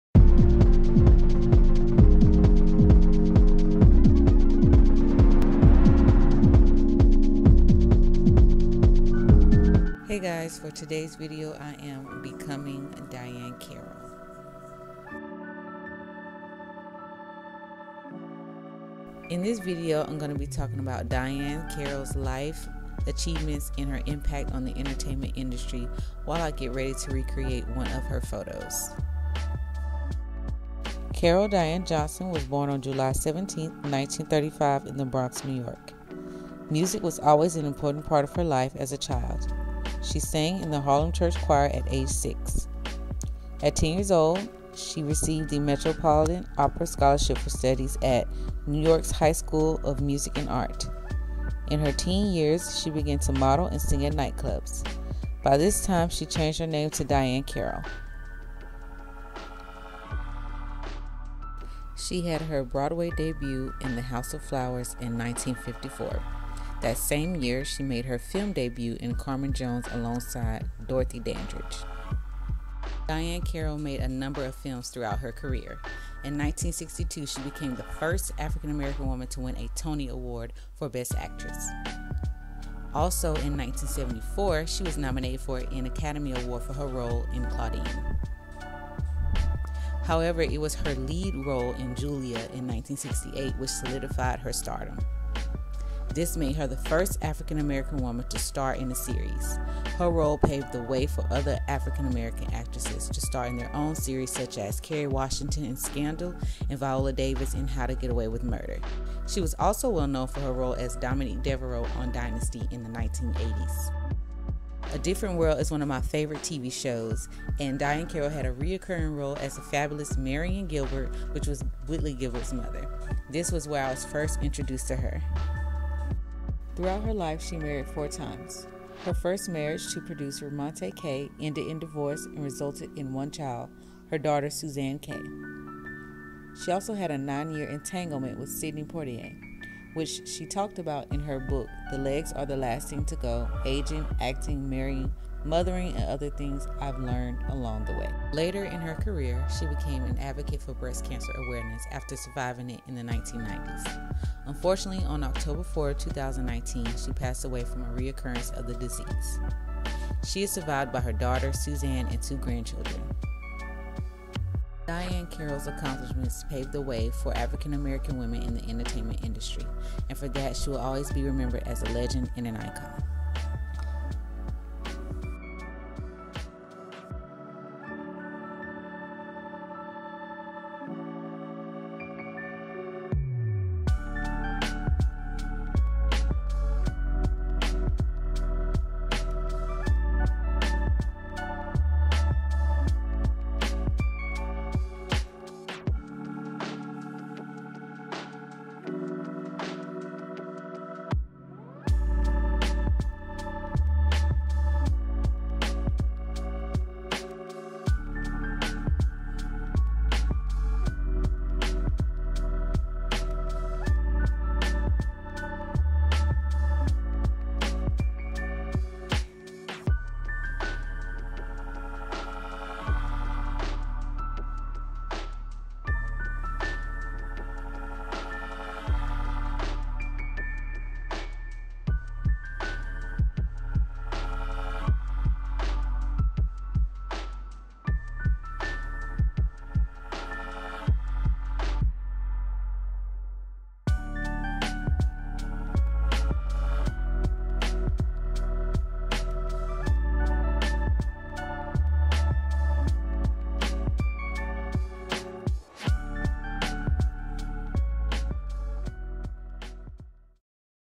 BECOMING DIAHANN CARROLL | BLACK HISTORY MONTH TRIBUTE
Hey BFFs! For today's video I wanted to do a Black History Month tribute for Diahann Carroll. I am a super fan of the show A Different World and this is where I was first introduced to her. In this video I will be getting ready to re-create one of her photos as I talk about her life, accomplishments, and her achievements in the entertainment industry.

FACEBOOK:  Not Your Average Mama